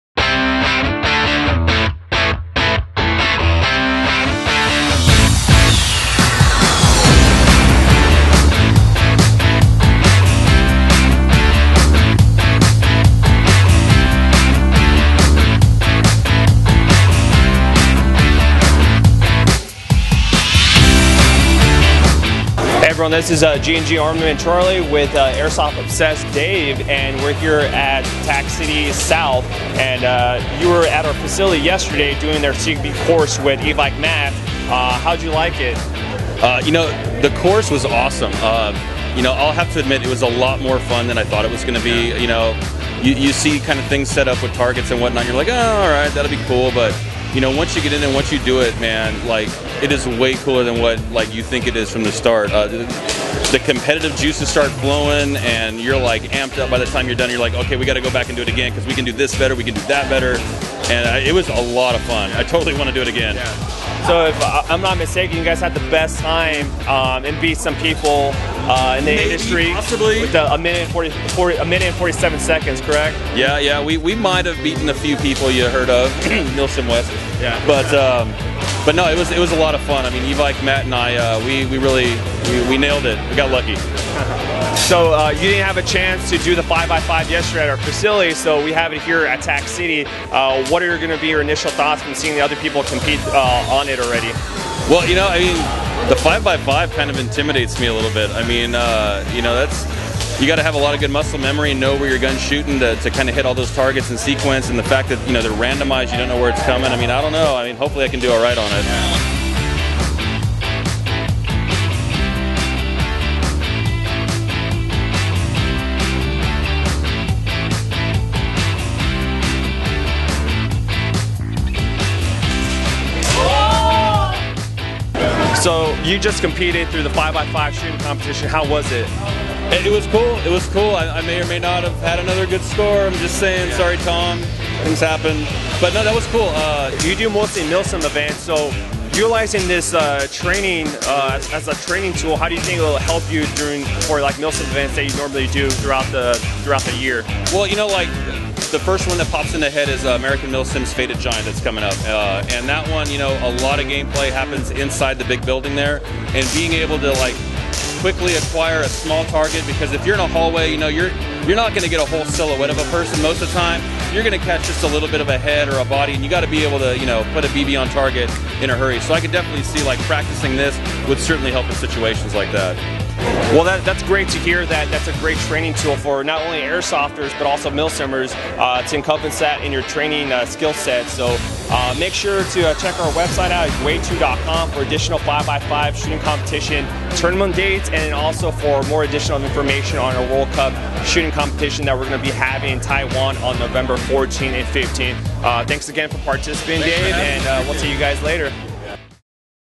Airsoft Obsessed 5x5 M.E.T. Shooting Competition
Dave from Airsoft Obsessed stopped by the G&G Appreciation game held at Tac-City South to compete in the 5x5 M.E.T. shooting competition.